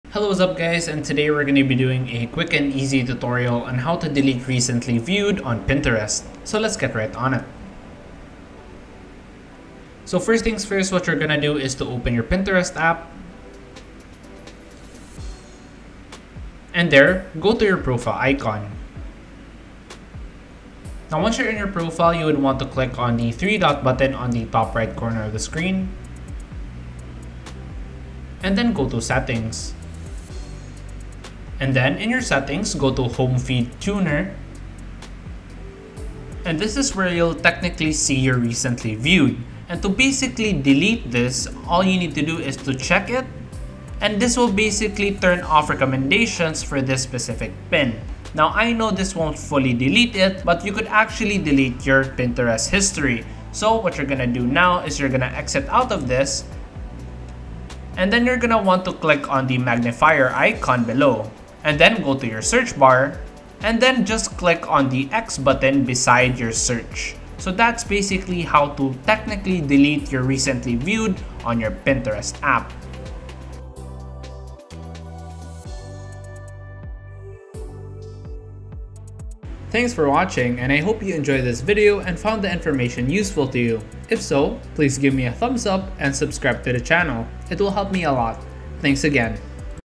How To Delete Quickly Recently Viewed On Pinterest.
In this short video, you will learn How to delete quickly recently viewed on Pinterest.